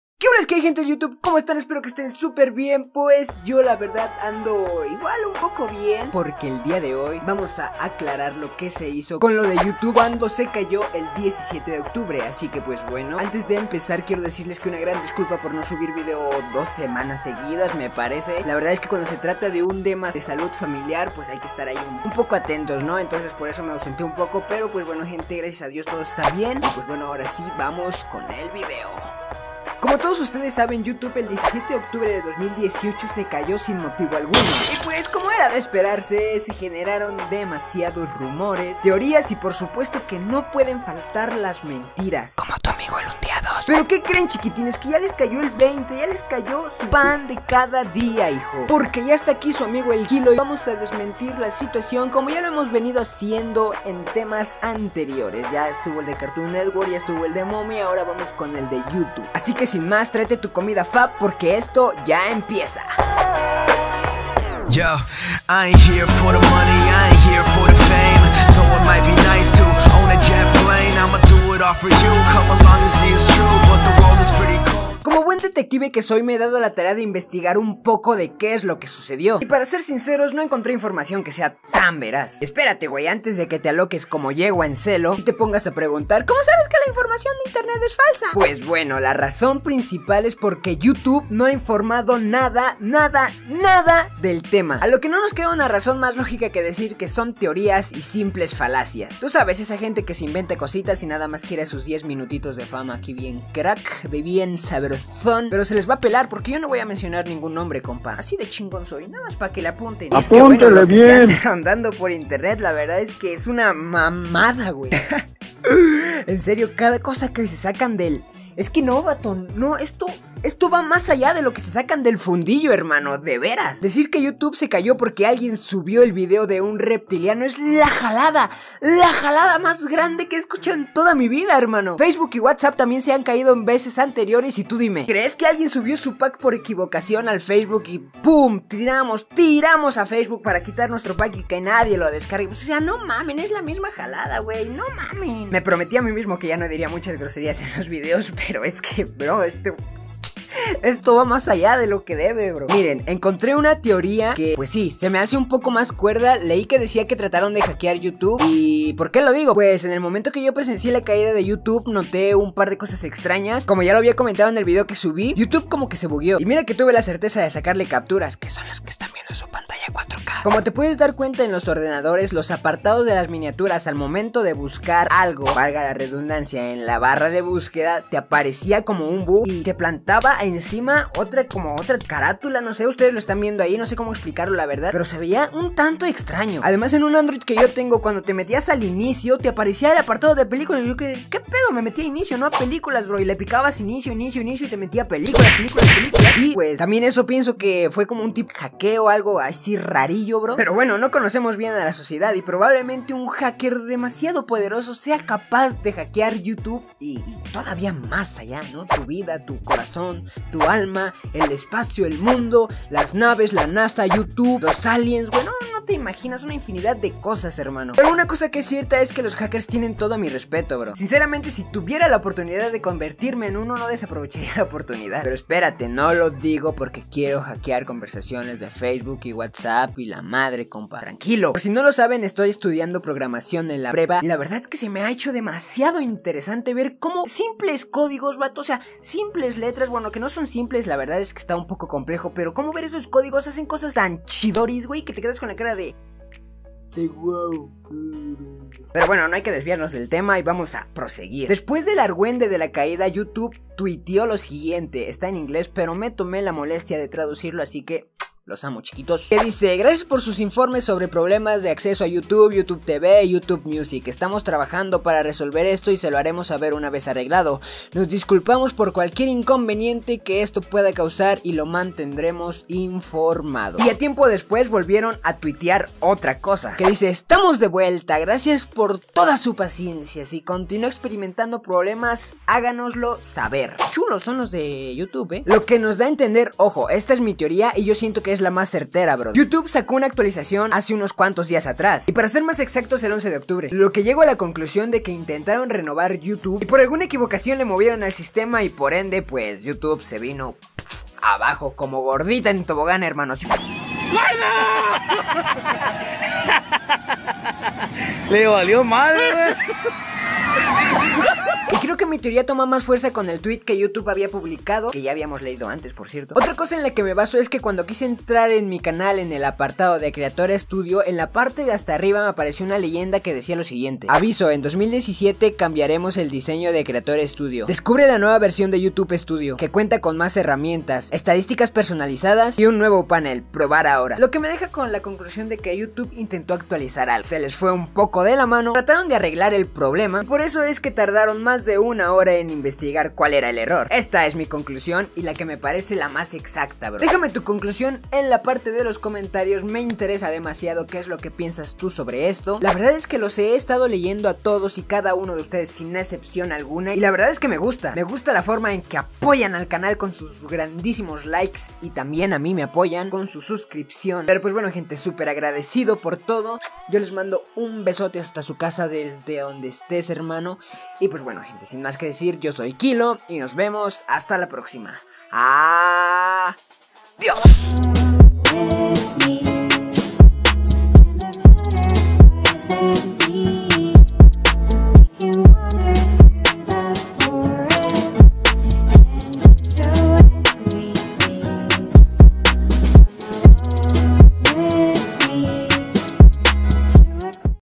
ESTA ES LA VERDADERA RAZON POR LA QUE YOUTUBE SE CAYÓ
Como todos sabemos YouTube se cayo a nivel mundial y como siempre suele pasar, se genero mucha polemica pero aqui esta su papi. Ya tocaba hacer un video sobre este tema y como hemos venido resolviendo los casos de cartoon network y momo.
ESPERO Y LES GUSTE, DEN A LIKE Y DISFRUTEN DEL WUEN VIDIO XD UWU